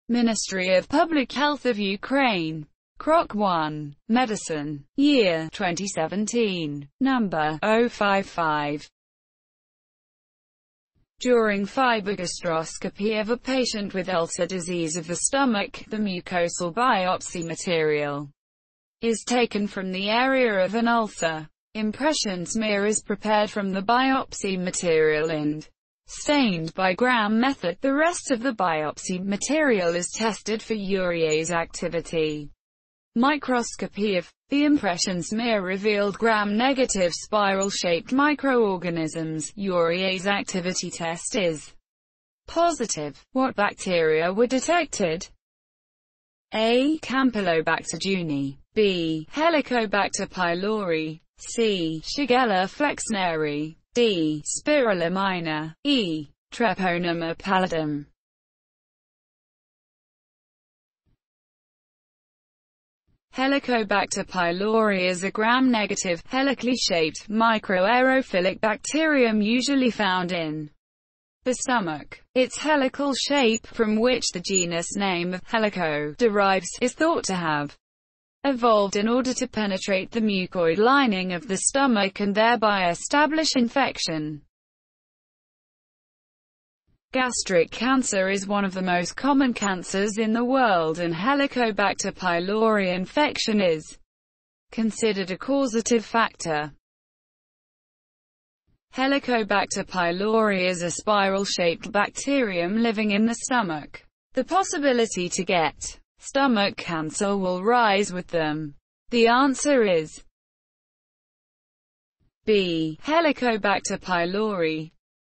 [ Krok 1 Medicine ] Year: 2017 - 055 (Ministry of Public Health of Ukraine)
During fibergastroscopy of a patient with ulcer disease of the stomach, the mucosal biopsy material is taken from the area of an ulcer. Impression smear is prepared from the biopsy material and stained by Gram method; the rest of the biopsy material is tested for urease activity. Microscopy of the impression smear revealed gram-negative spiral-shaped microorganisms, urease activity test is positive. What bacteria were detected?

Campylobacter jeuni
Helycobacter pylori
Shigella flexneri
Spirilla minor
Treponema pallidum